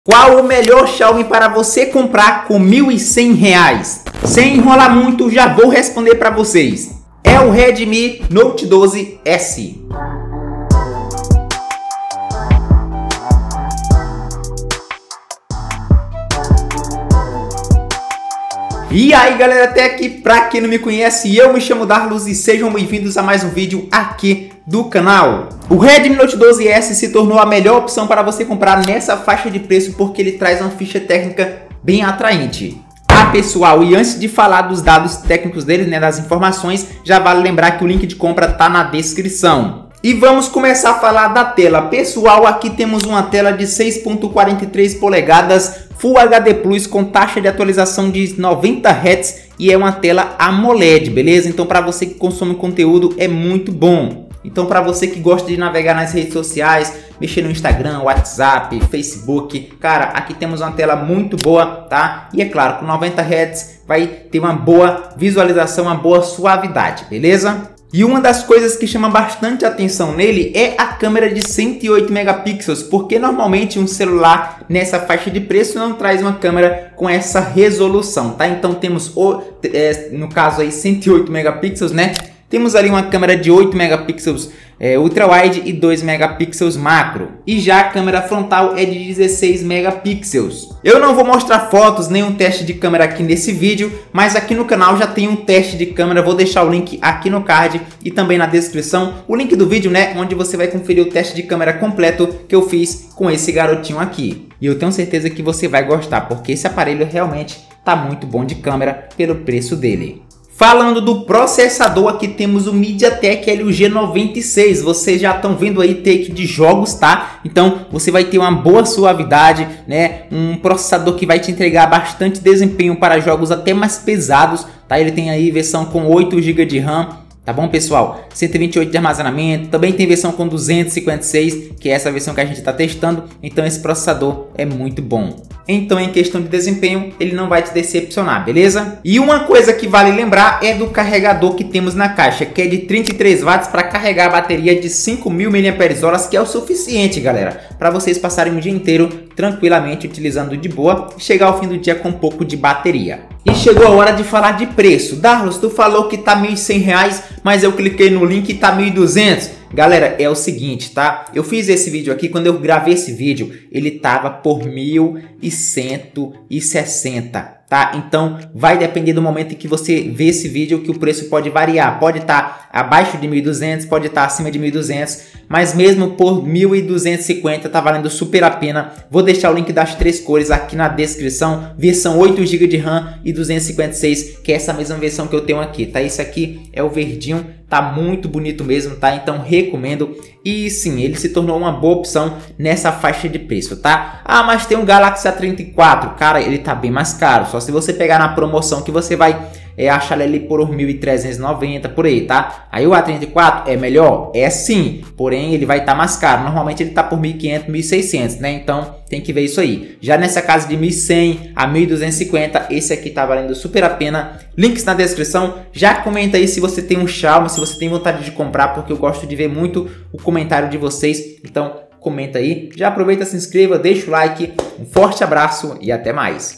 O Redmi Note 12S é o melhor XIAOMI Custo-Benefício de 2023
QUAL O MELHOR XIAOMI PARA COMPRAR COM 1100 REAIS? SEM ENROLAR VOU RESPONDER LOGO DE CARA, É O REDMI NOTE ...